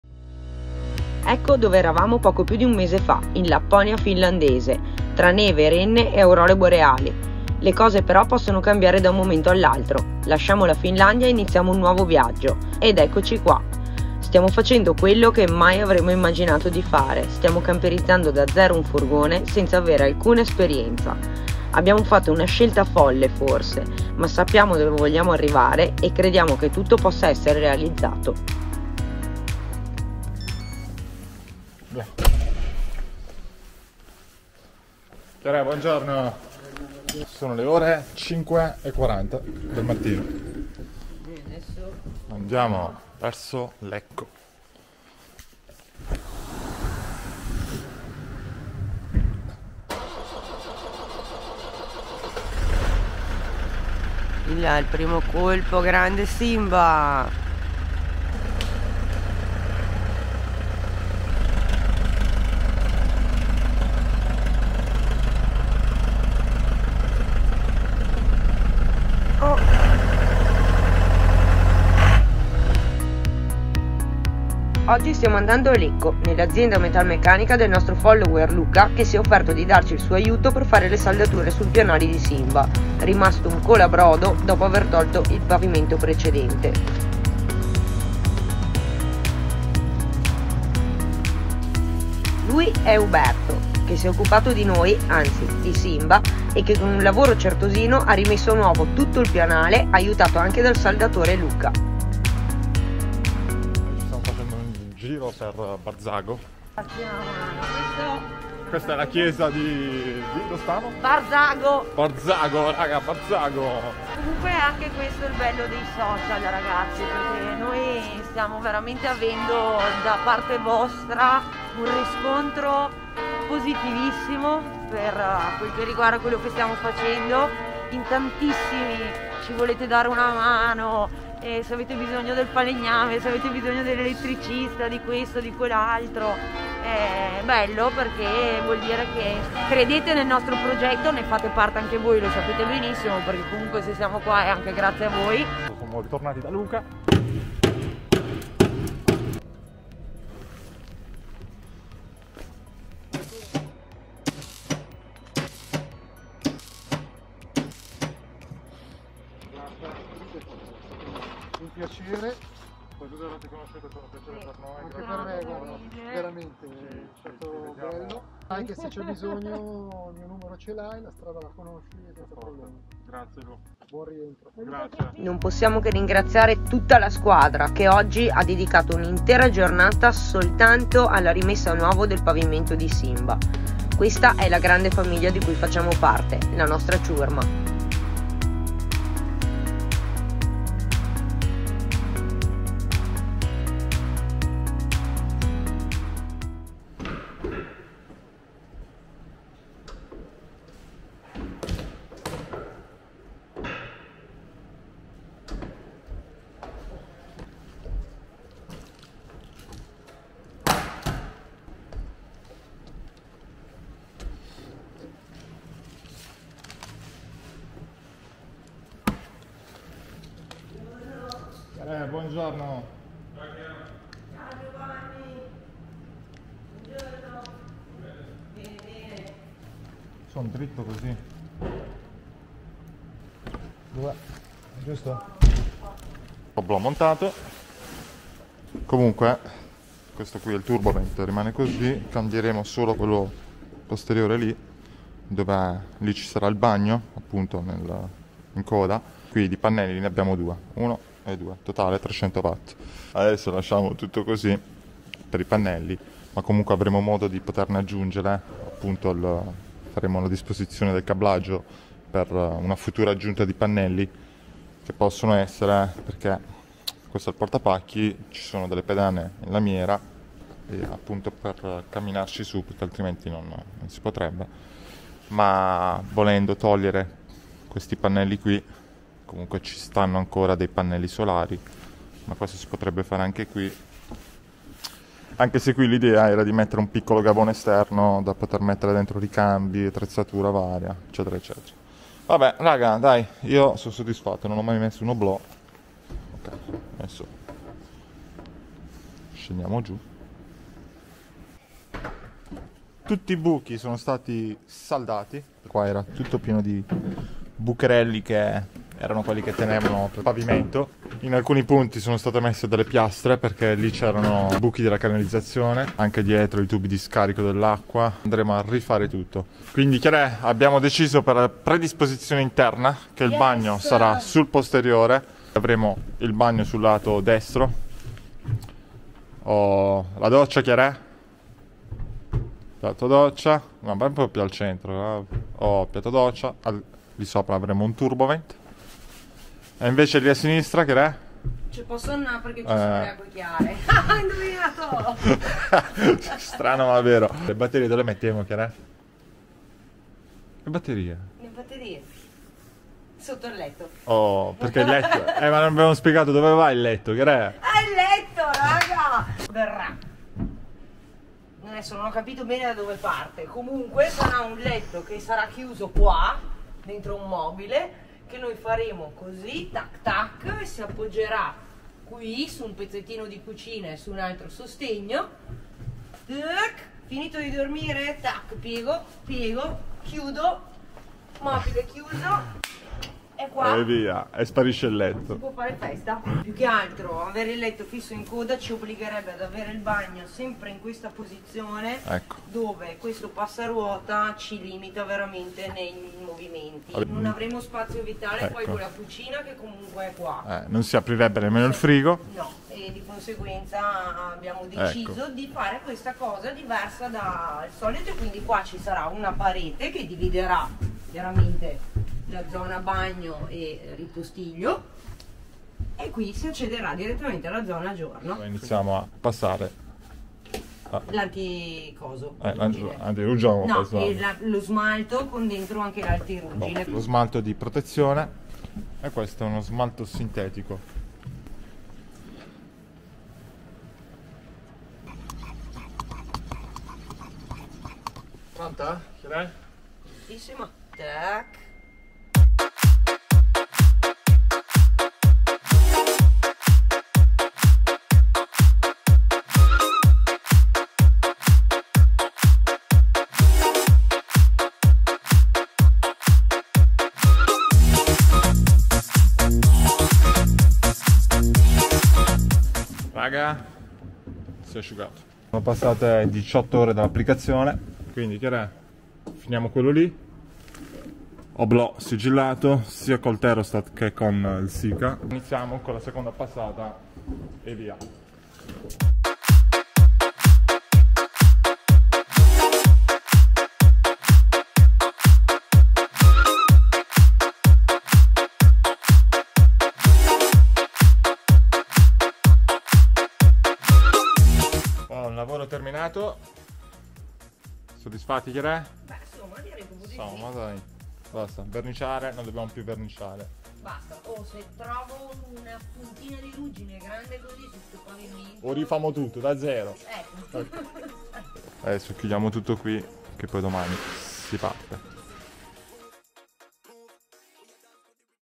RESTAURO PIANALE 🔥👉🏻 CAMPERIZZAZIONE DAILY 4X4 🌍 EP.6
Partiamo per Lecco, dove ci aspetta Luca, un nostro carissimo follower, nella sua azienda metalmeccanica con una squadra di saldatori pronti a chiudere tutti i buchi rimasti sul pianale di Simba dopo aver tolto il pavimento dell' allestimento precedente.
Questo progetto non è solo nostro, ma di tutte le persone che fino ad oggi hanno creduto e continuano a credere in quello che stiamo facendo ed è sopratutto grazie a loro che, passo dopo passo, la meta si fa sempre più vicina!
Questa è la Nostra Ciurma! 🔥

🚐 AMAZON SHOP 🚐

Per seguire le nostre avventure di tutti i giorni! 🌍